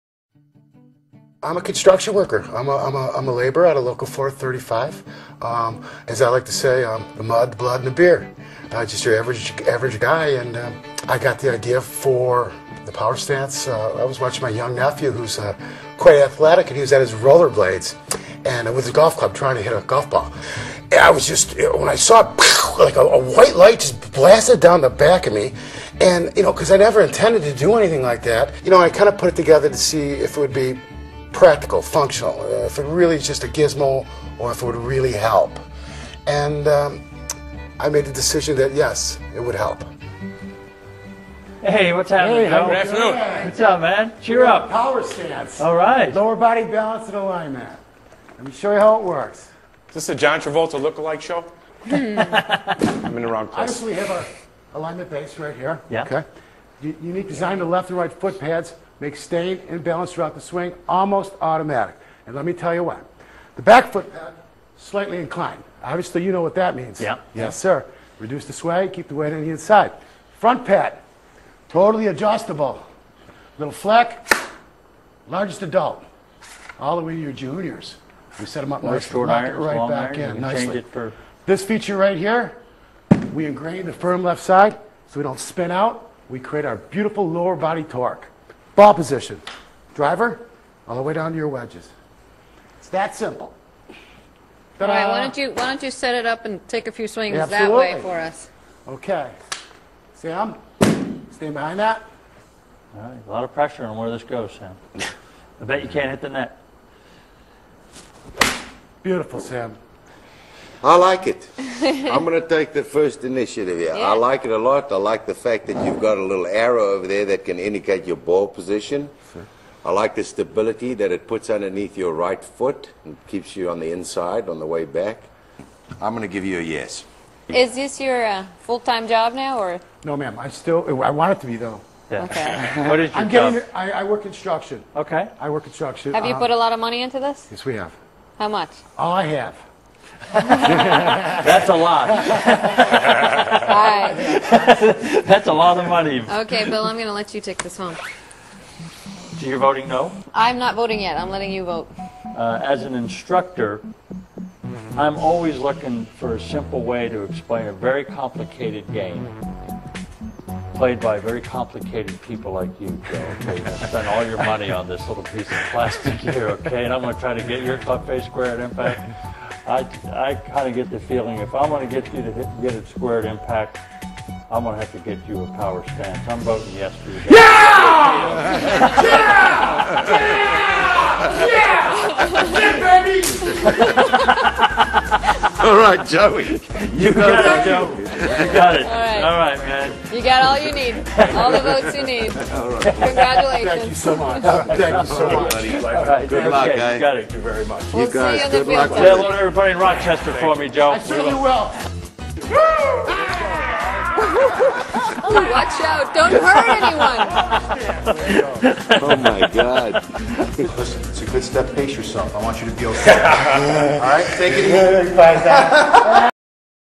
Power Stance Golf Training Aid at
Fewer things are more important in generating distance and accuracy than balance and body position. And nothing is more effective at teaching proper balance and position than the Power Stance Trainer the most comprehensive lower body trainer. When your body and feet are in proper position and in balance, you are poised to get the most from your golf swing.